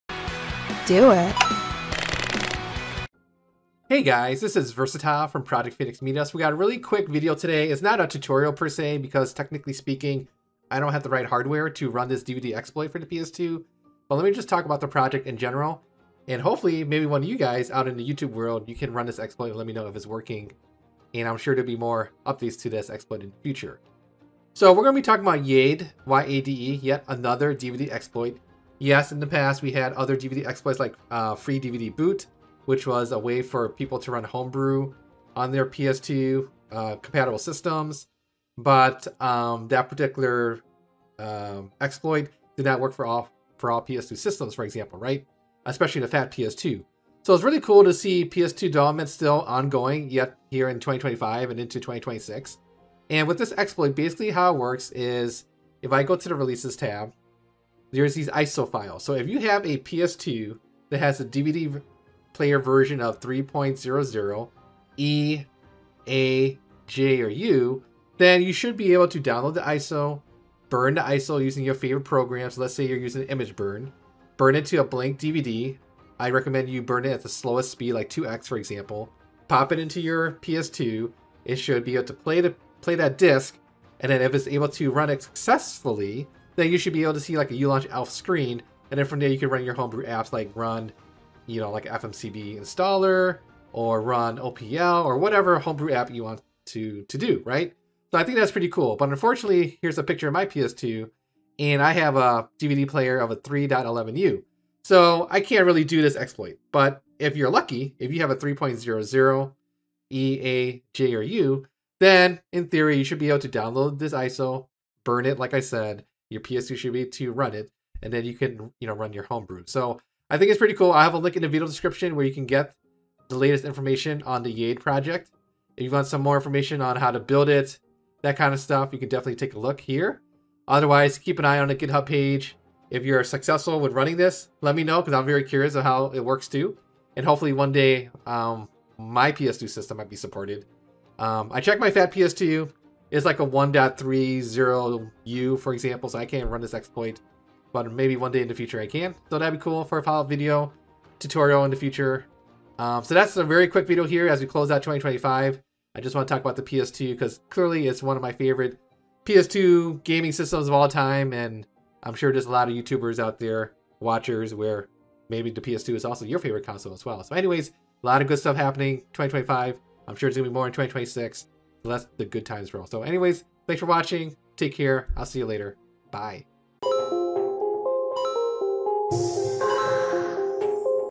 New PS2 DVD Exploit - YADE (2025, English)
Today's video I go over what is YADE, which is a brand new dvd exploit for the PS2 so if you have a DVD player version of 3.00U,A, J, or E, then you should be able to run the burnt disc image into UlaunchELF, and then run homebrew of your choice.

Could be a great alternative to FreeDVD Boot. See links for all latest developments of this great project.

Needs a VPN? We recommend Private Internet Access (PIA)

MoNeYINPHX PC Specs